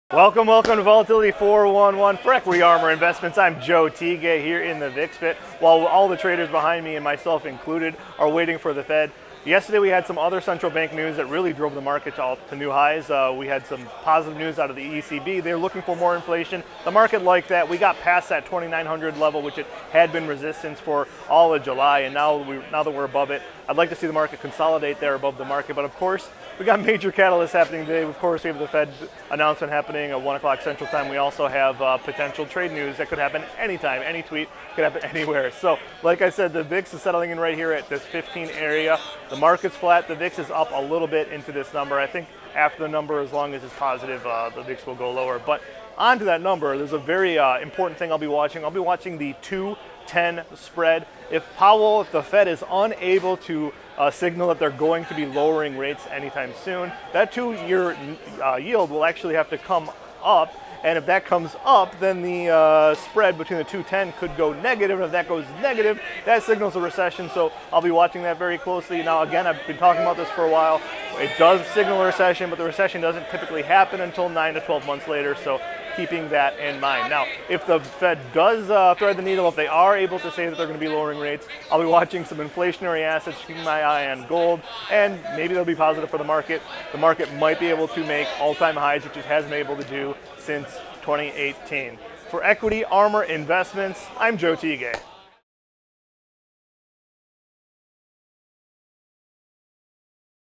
Fed Announcement Today
Ahead of the Fed announcement later today, Joe Tigay talks the 10-2 Year Treasury Yield Spread, inflationary assets, central banks & other market movers & volatility catalysts.